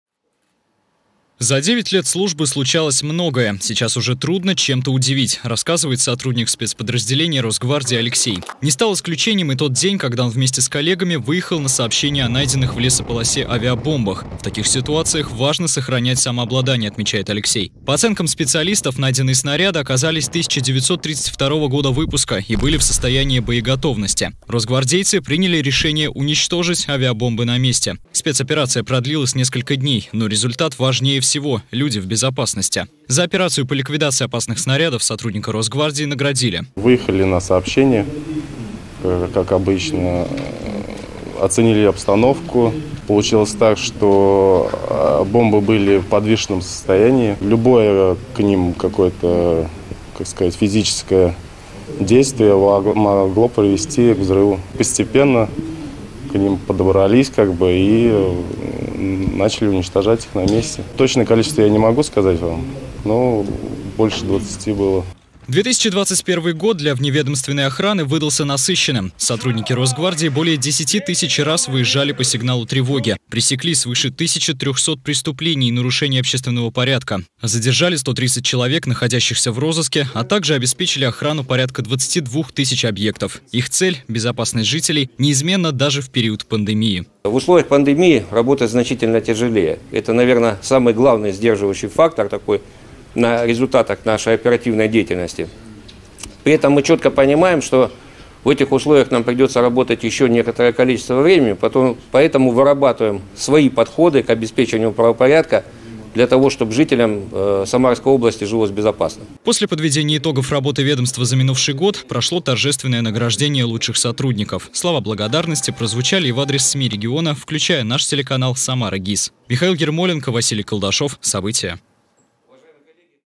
В самарском управлении Росгвардии подвели итоги работы за 2021 год
Значительный вклад в укрепление правопорядка, борьба с терроризмом и безопасность самарцев — их задачи не меняются. В управлении Росгвардии по Самарской области подвели итоги работы за 2021 год. Подробнее о проделанной работе росгвардейцев расскажет Михаил Ермоленко.